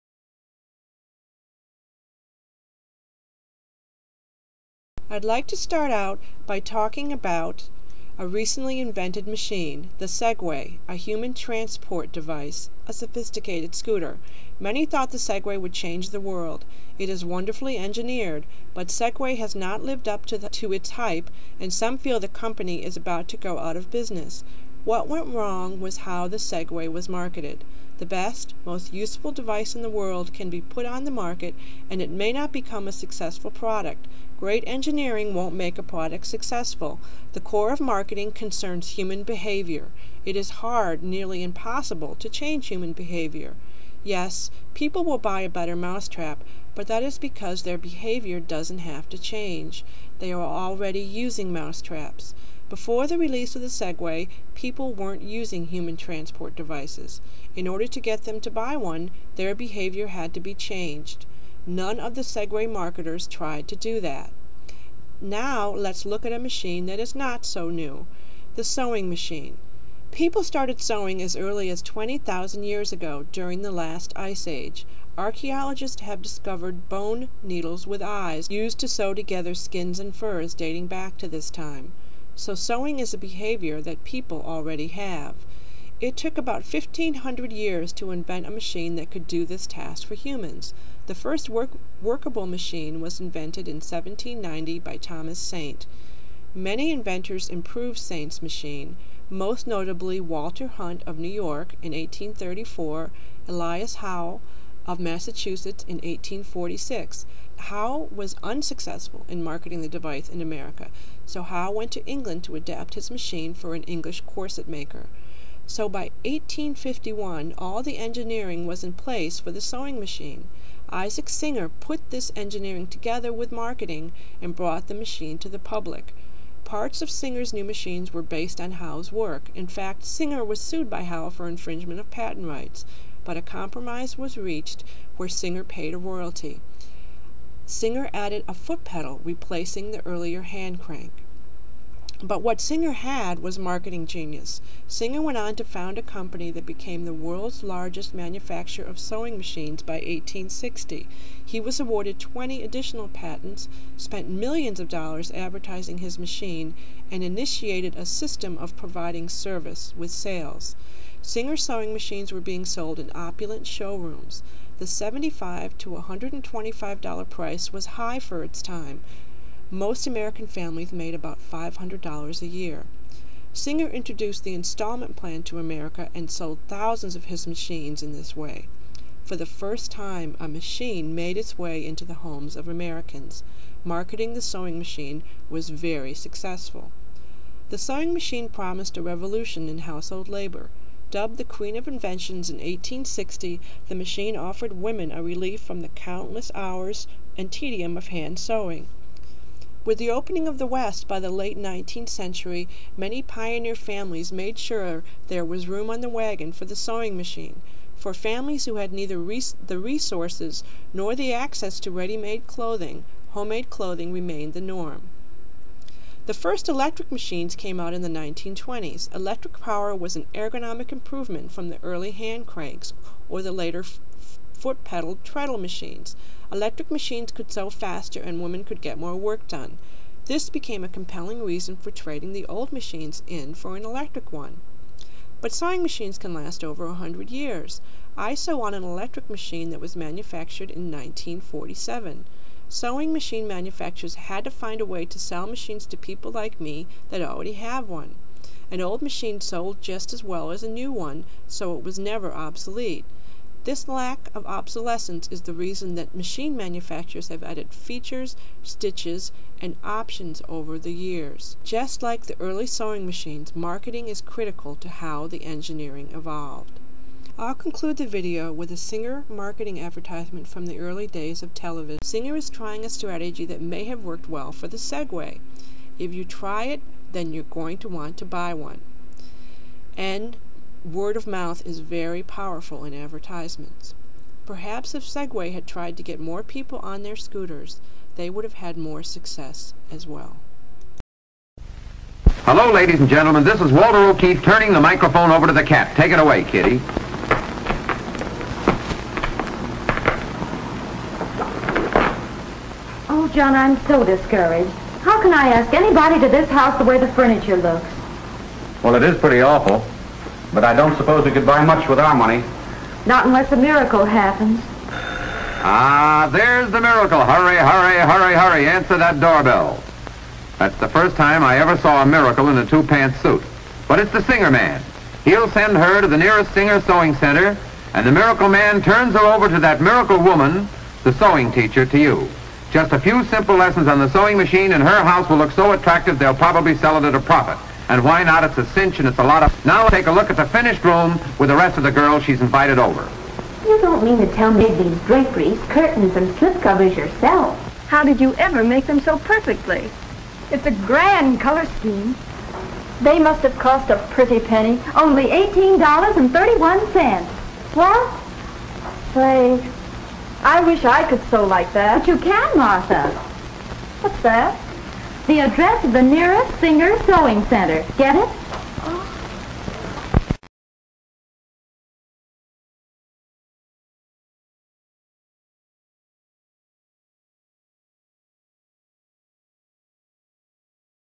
The Marketing of the Sewing Machine vs Segway
This is a project for my human/machine class.  I show how sewing machine marketing drives the engineering of the machine.  I contrast it to the segway, where engineering drove the marketing.